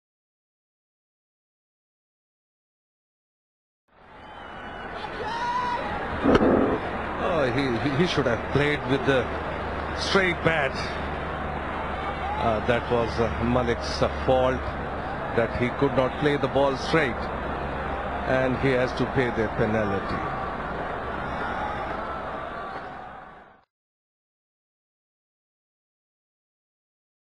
Albie & Makhaya: *Bear Hug*
Albie Morkel and Makhaya Ntini celebrate after Ntini gets Shoaib Malik.

I thought this was really cute.